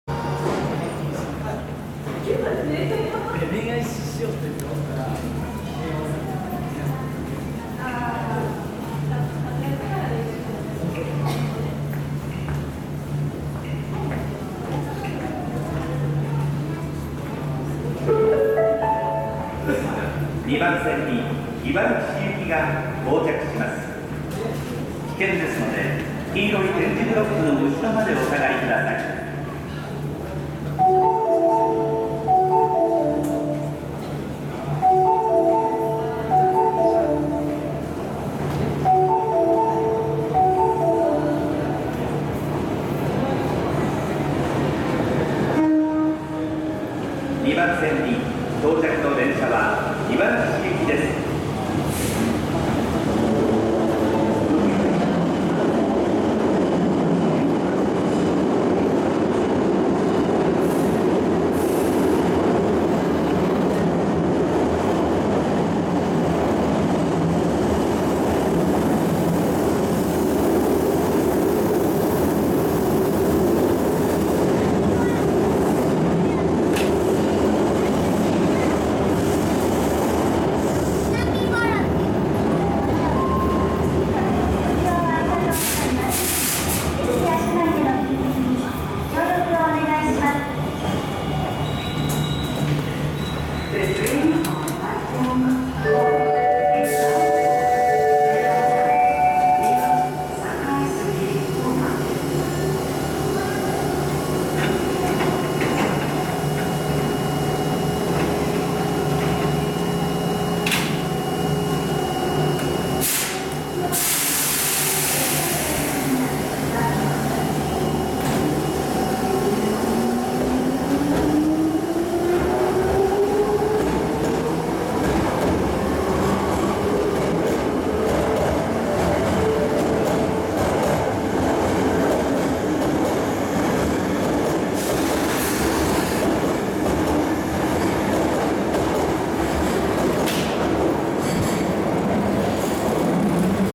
【放送更新】大阪市営地下鉄 堺筋線の接近放送も変わりました 男性編[茨木市行]
阪急と相互乗り入れを行っている堺筋線も放送が更新されていました。
谷町線・四つ橋線同様「黄色い点字ブロックの後ろまでお下がりください」の文言が追加されました。